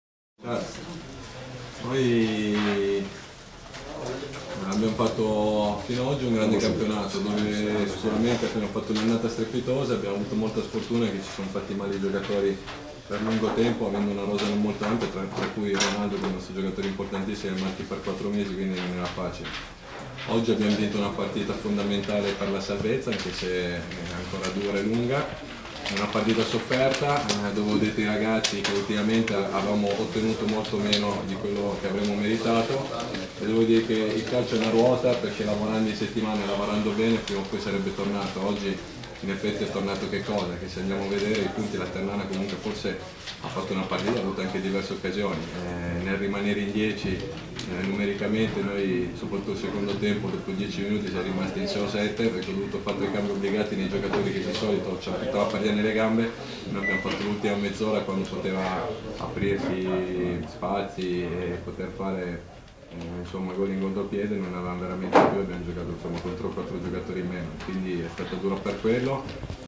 Ternana - Pro Vercelli 0-1 Il commento di Cristiano Scazzola
Terni, il commento del tecnico della Pro Vercelli a fine partita